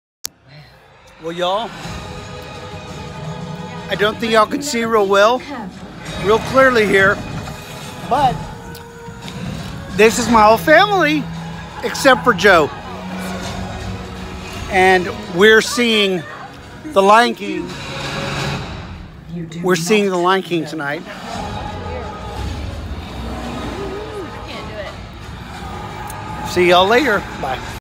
REGAL 14 MOVIE NIGHT W/MY FAMILY🎉🎉🎉🎉🎉
We all came to see the Lion King tonight🤣🤣🤣🤣🤣🤣🤣🤣🤣